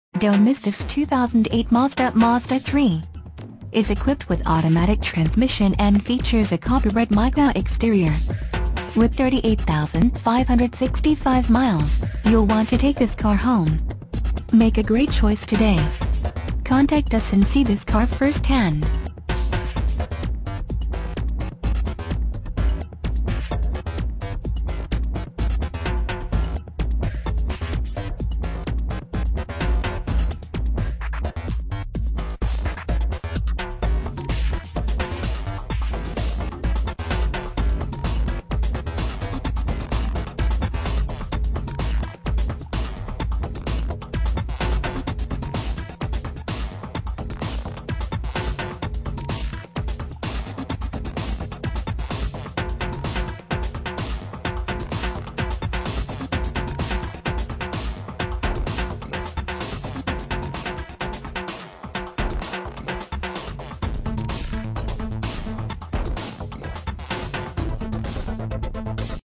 2008 MAZDA MAZDA3 Cleveland, GA
2008 MAZDA MAZDA3 Cleveland, GA
Stock

When everything seems to come together perfectly in a car, automakers have very little incentive to mess with success. So for its fifth year running, the 2008 MAZDA3 fore-goes any major changes to its lineup, looks, or layout. Side curtain airbags have been added to the front seats. Also new, the i Touring now mirrors the s Touring in terms of equipment, receiving ABS brakes, fog lights, leather trim, and 17-inch wheels. The '08 MAZDA3 distinguishes itself from the similarly priced Civic by offering more power, wider dimensions, and more precise handling, but lags significantly behind its biggest competition in fuel economy.  Unique styling also sets the MAZDA3 apart from the crowd, particularly in the cool and funky hatchback, with its athletic stance and chiseled lines. Add to that the hatchback's standard rear spoiler, side sills, and body-colored grille, and this compact wagon aspires to and attains much sportier abilities than its practical cargo-carrier dimensions might indicate.   This vehicle combines sportiness yet practical , with it hatchback that could fit big objects with no problem. Come in today and see for yourself.